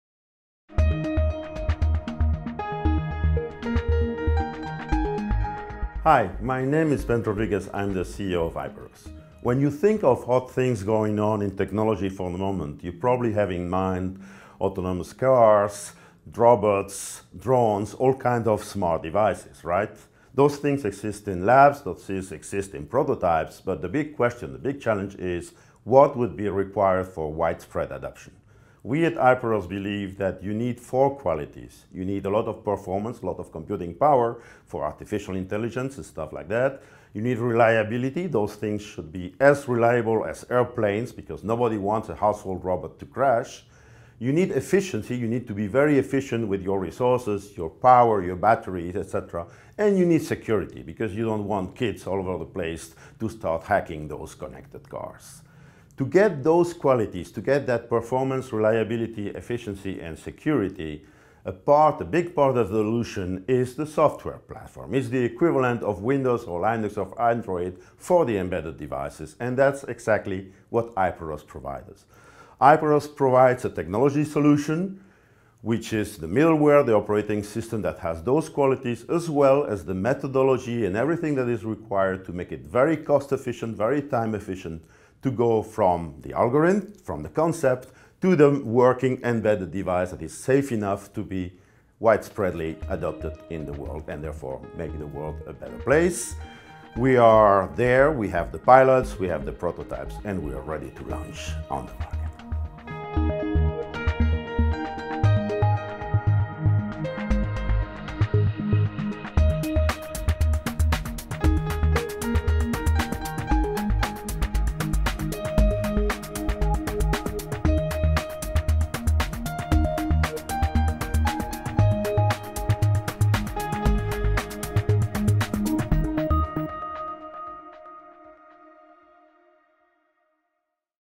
HIPPEROS: EuroCPS H2020
As part of the EuroCPS H2020 initiative to foster innovative European SMEs in the area of CPS, a few selected companies were invited to pitch their project. In this video the HIPPEROS CEO pitches the company mission of Intelligent Autonomy with High Performance Embedded Systems.